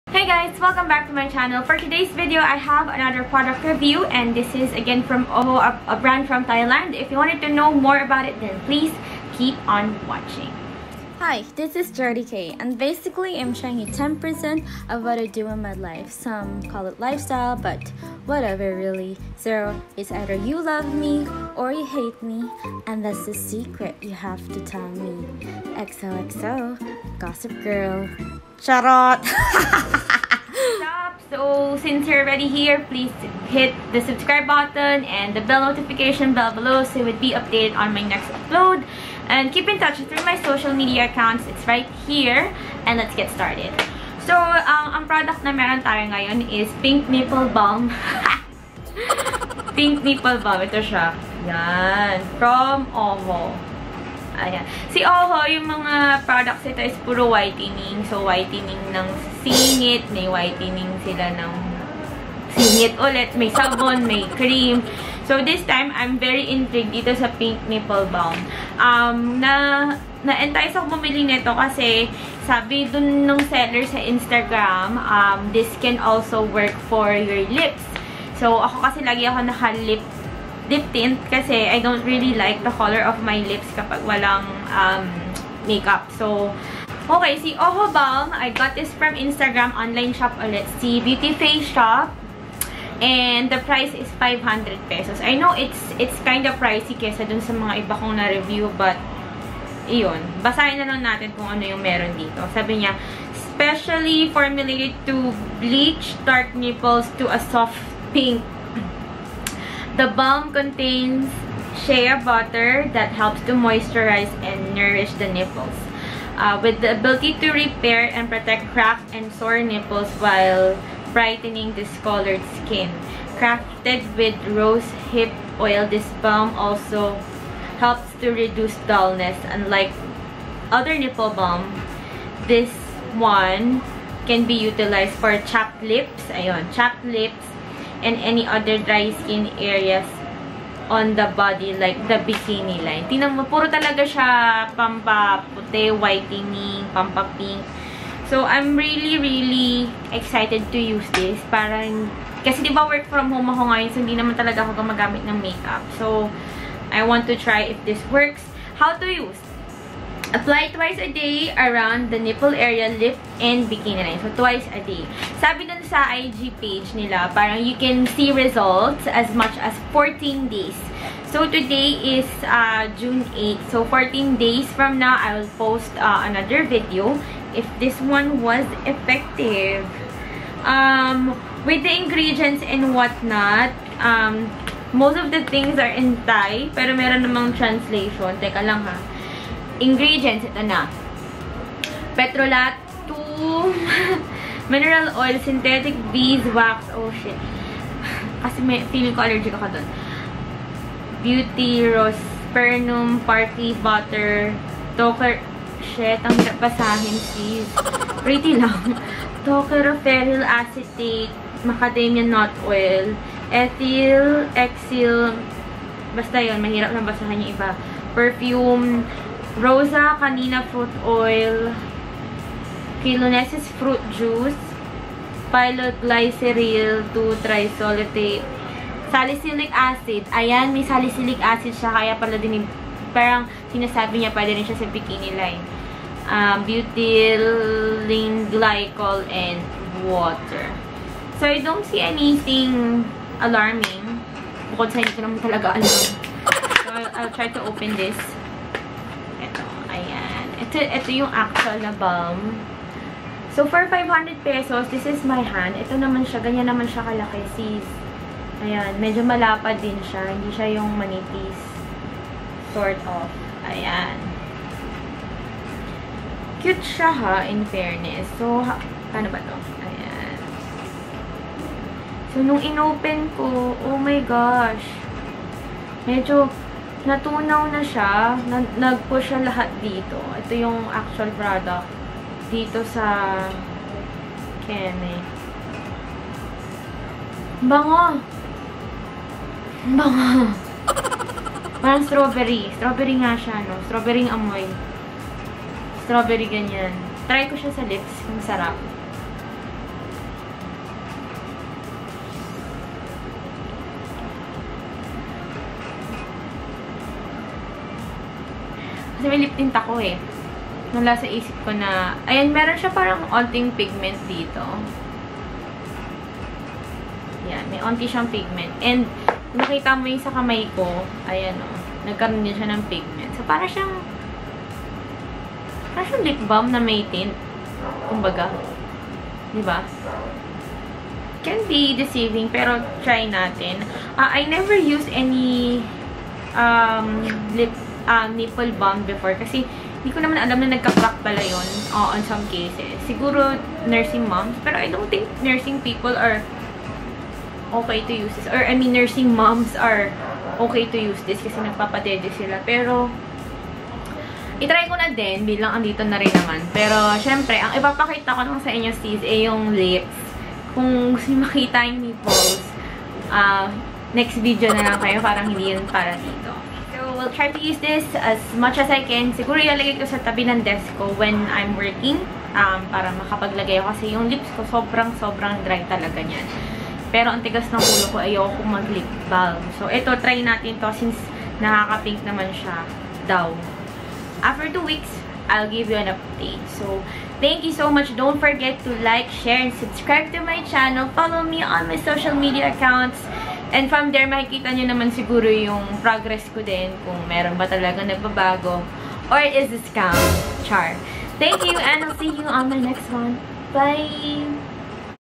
OHO NIPPLE BALM REVIEW
Hi! You’re here!! Lol
OHO Nipple Balm first impression review
500php bought from ig: Beautyfaceshoppe

Will be uploading update next week!!

Xo 😘